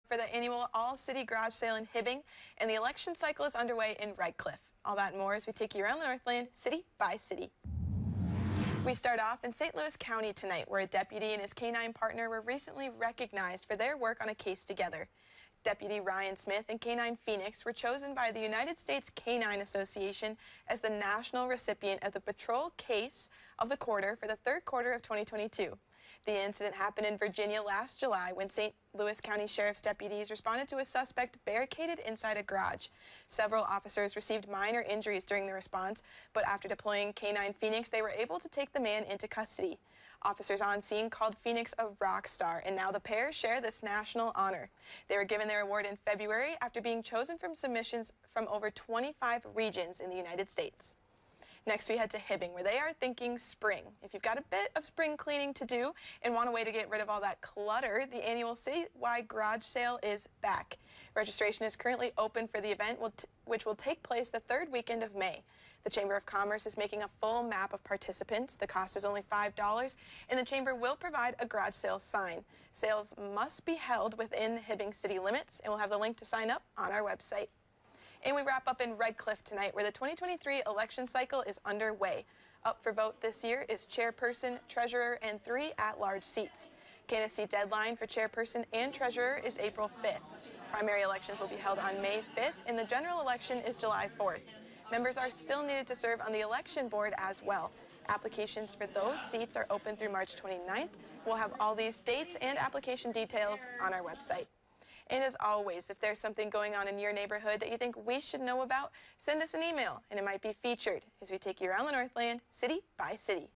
City by City: St. Louis County, Hibbing, Red Cliff
A St. Louis County Deputy and his K9 partner recently received a national honor.